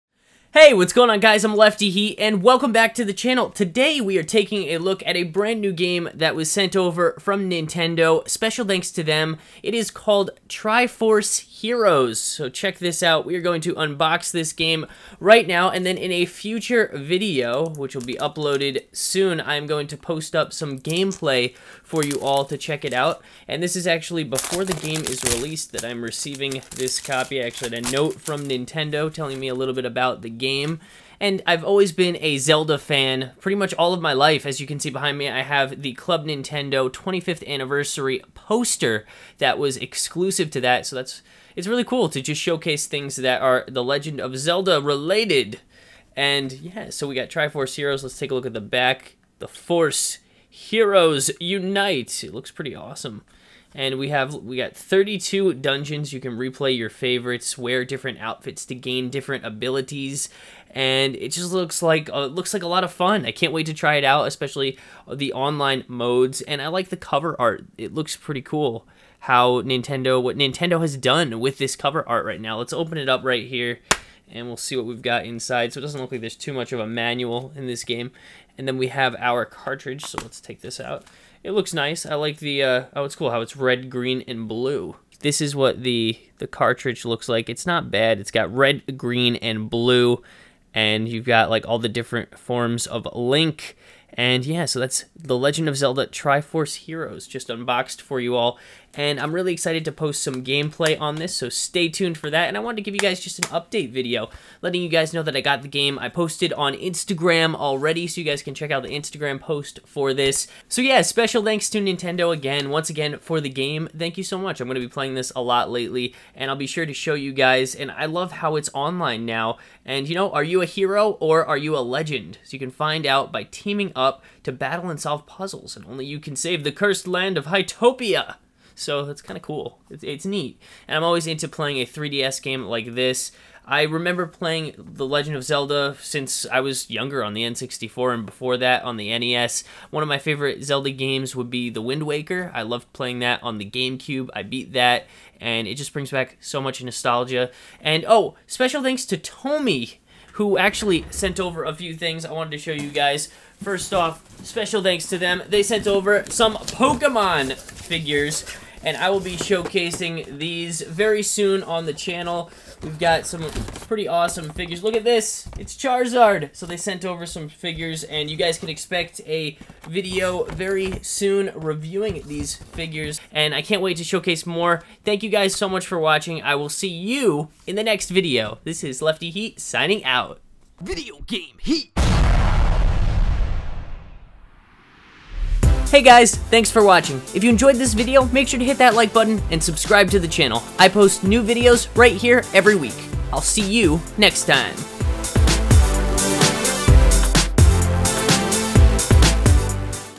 The Legend of Zelda: Triforce Heroes Unboxing, 3DS Game Launch, Update, New Online Modes
The Legend of Zelda: Triforce Heroes Unboxing, 3DS Game Launch, Update, New Online Modes, Features - In this video LeftyHeat Unboxes the brand new game Triforce Hero's that he received from Nintendo ► Make sure to SMASH that "Like" button and get ready for a new videos every week.